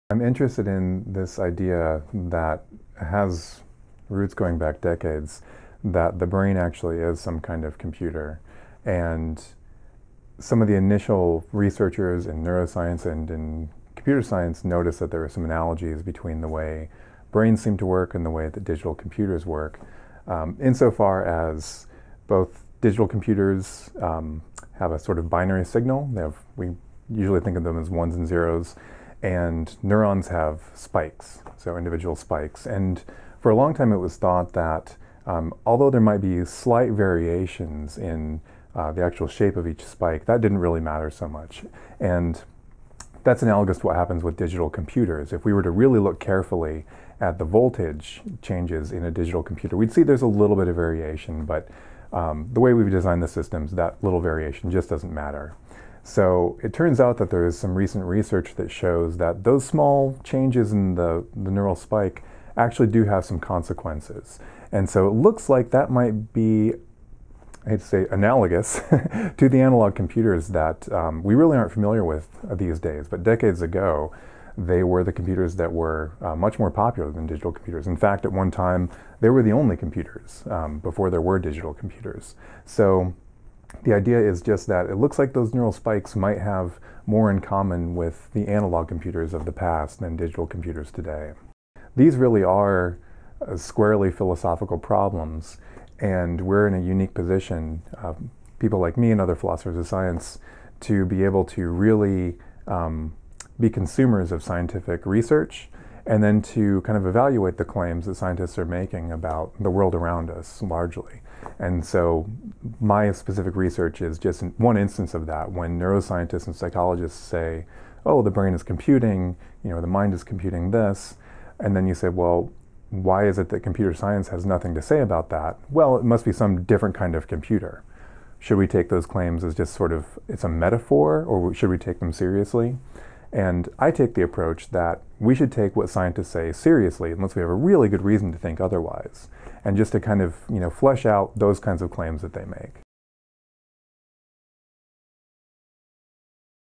If our brains are like computers, they're analog, philosopher says
Corey Maley, assistant professor of philosophy at the University of Kansas, has received a $155,121 National Science Foundation Scholar Award to work on a book manuscript detailing how some neural processes might be analog computations. The grant will allow Maley to travel to academic conferences featuring research on computational neuroscience, cognitive science and philosophy of science to discuss this type of research with experts from a wide variety of disciplines.